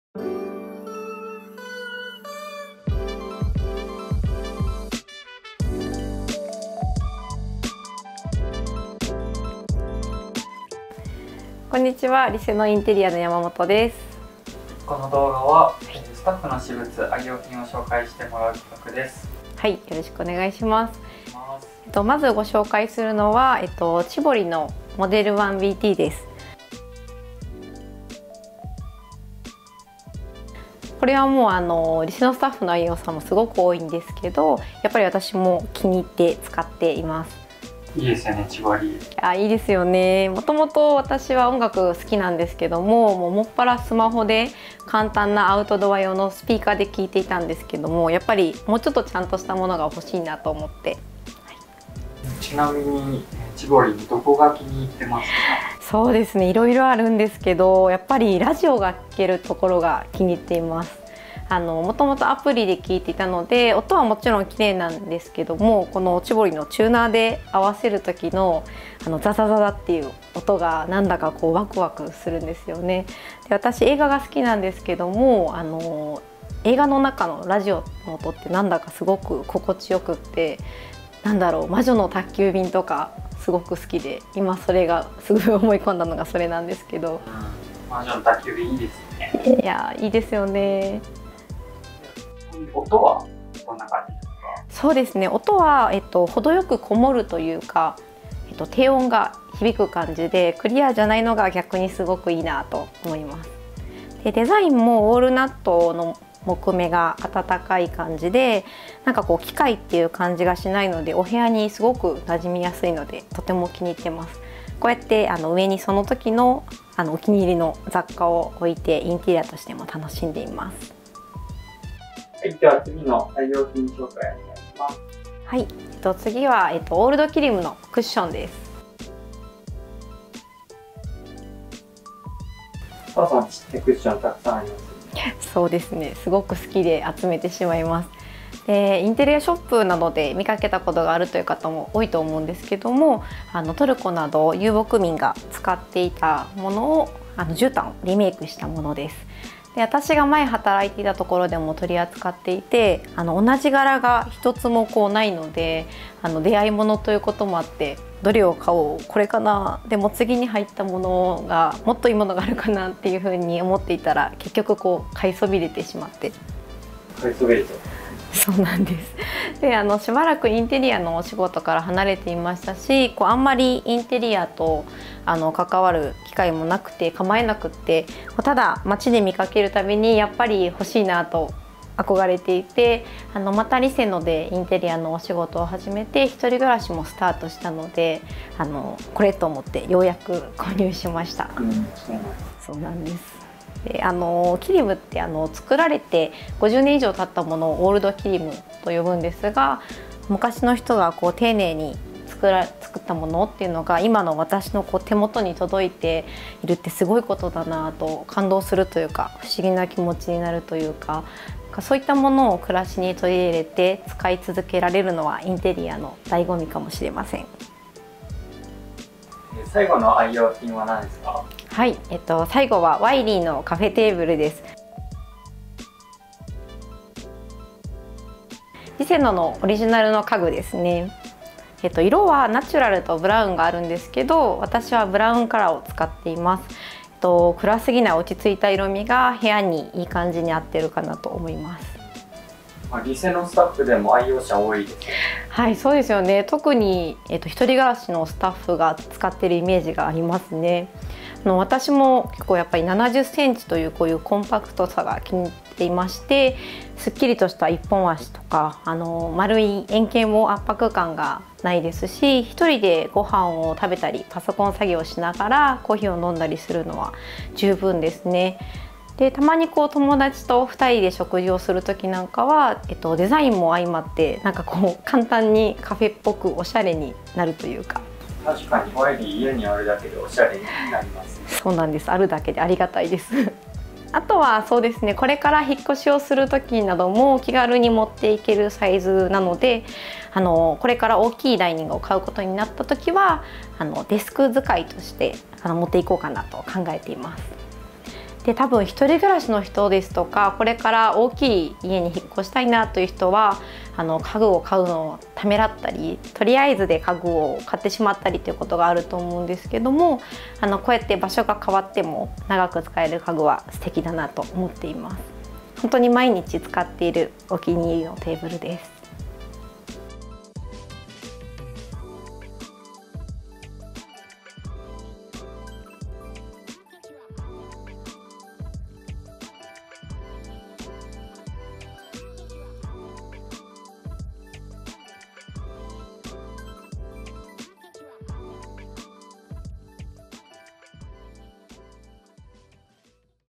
【インテリア愛用品】買ってよかったアイテム3選 エディター山本
スタッフのお部屋にお邪魔して、
「本当に購入してよかった」と思える
愛用品を紹介していく今回の企画。

第2回目は、商品アップ業務や
ブログライティングを務める制作部 山本。

自分の「好き」という気持ちを軸に
インテリアを楽しんでいるという彼女。

そんな彼女のお気に入りの愛用品たちを
紹介してもらいました。

ぜひ楽しみながらご覧ください＾＾

■クッションカバー オールドキリム

0:00 オープニング
0:21 オーディオ
2:01 テーブル
3:39 クッションカバー
5:20 エンディング